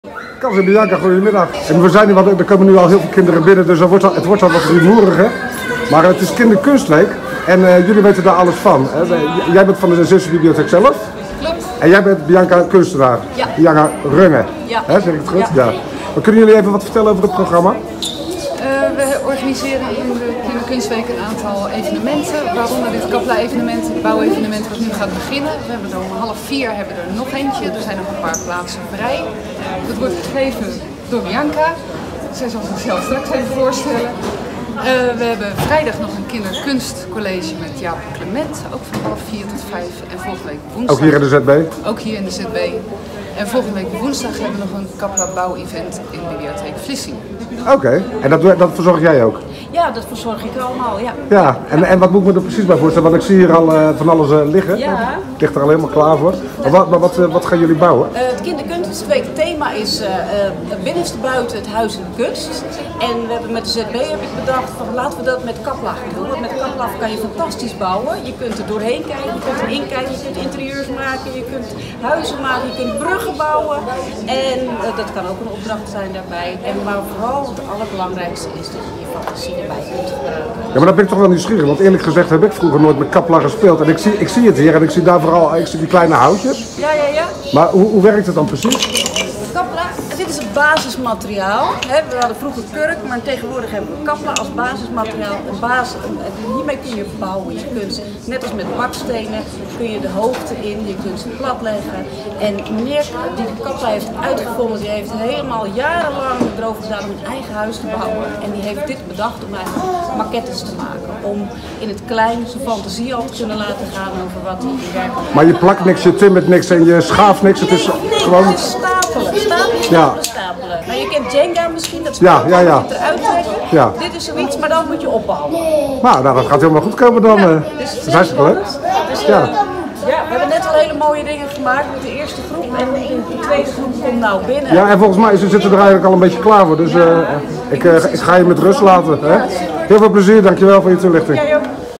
Cas en Bianca over de Kinderkunstweek in de ZB 11-22 maart 2020
Wat een leuke reacties kregen ze, op het Kapla Bouw-event! Dat is altijd fijn om te lezen. En het was ook echt leuk. Hebben jij en je kinderen het gemist? Dan is er nog een herkansing in ZB Bibliotheek Vlissingen, op woensdag 18 maart! In dit filmpje vertellen Cas de Nood en kunstenares Bianca Runge er meer over.

Maar er is natuurlijk nog veel meer te doen tijdens de Kinderkunstweek. Aanstaande dinsdag het evenement Klokhuis Meet Up 'Bouwen in de toekomst, bijvoorbeeld en aanstaande vrijdag Kinderkunstcollege en Expo. Ze draaien het thema van de week lekker om en zetten beeldende kunst die ze zelf in huis hebben in het zonnetje, voor alle kunstliefhebbers, voor kinderen van 6 tot 12 jaar en hun ouders.

Kom je ook kijken? En luisteren? Kinderkunstspecialist Jaap Clement is aanwezig om je alles te vertellen wat je maar wilt weten over (onze) kunst.